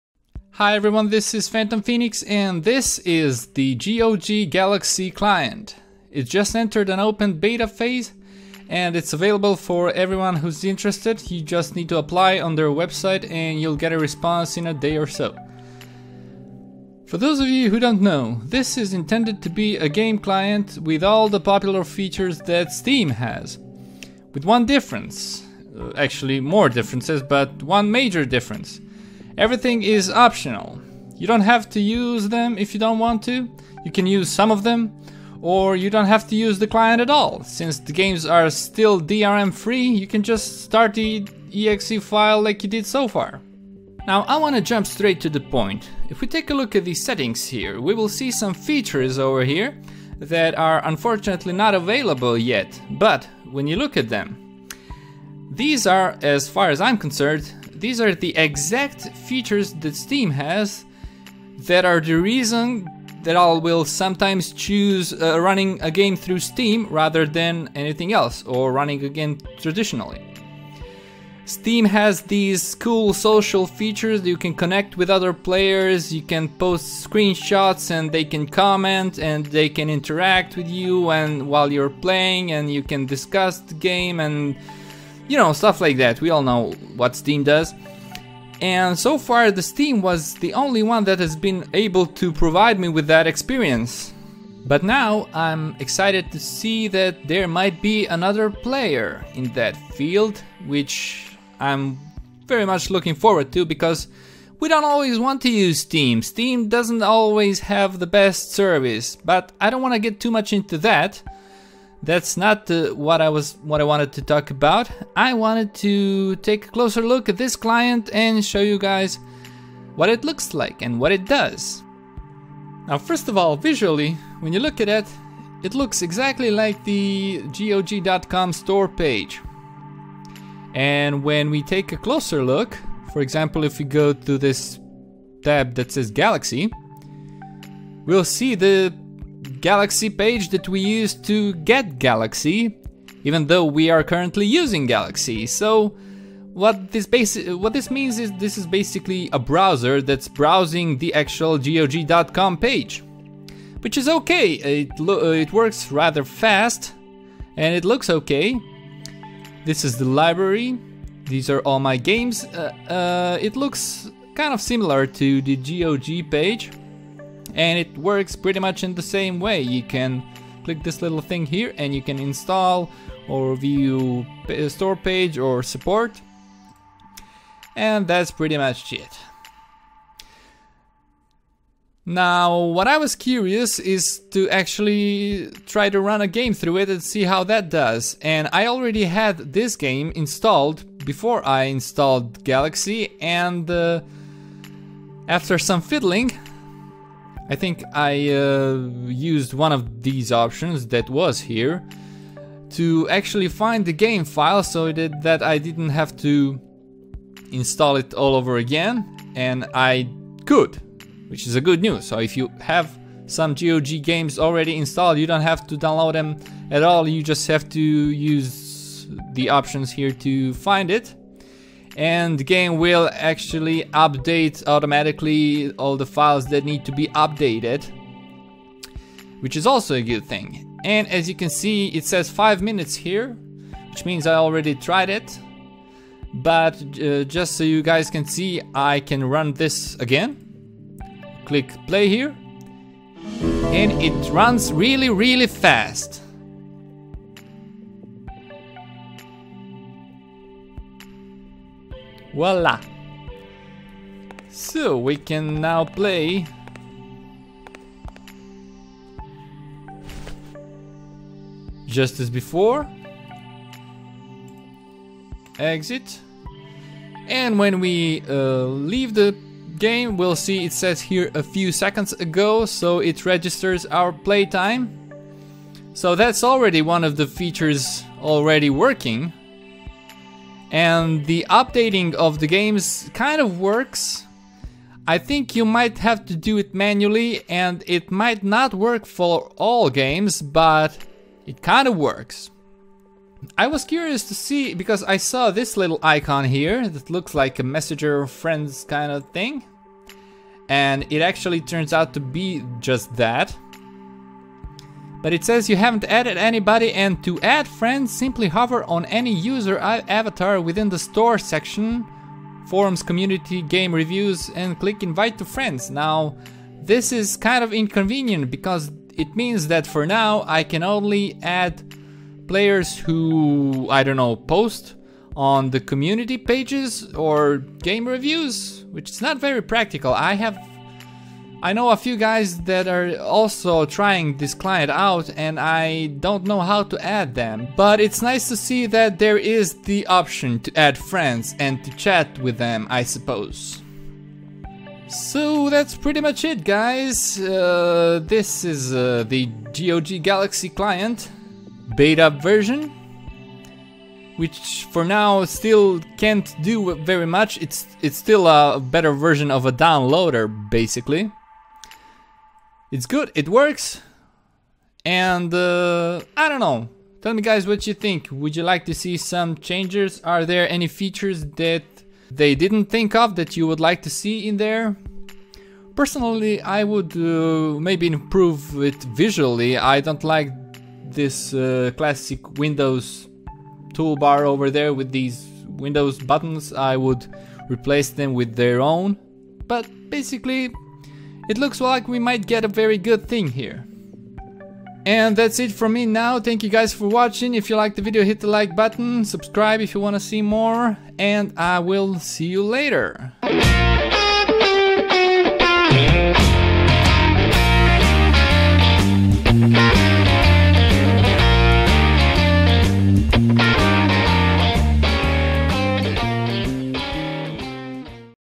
My first look into the GOG Galaxy client (beta)
GOG Galaxy client is in open beta phase, this is my first look into it.

0:15 - What is GOG Galaxy?
0:43 - Main features (coming soon?)
2:00 - Visual overview and browsing
3:00 - Starting an installed game through Galaxy
4:50 - Friends section
5:45 - Summation